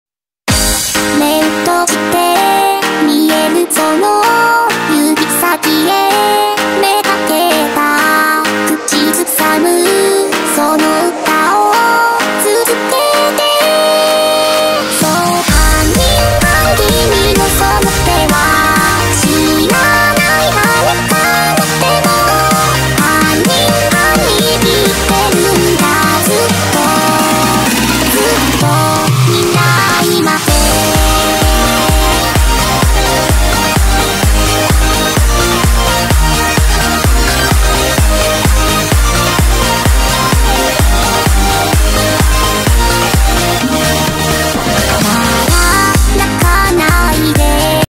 Abematvにでかミクちゃん(初音ミク)と出演しました！
近藤顕彦さんも出演しています。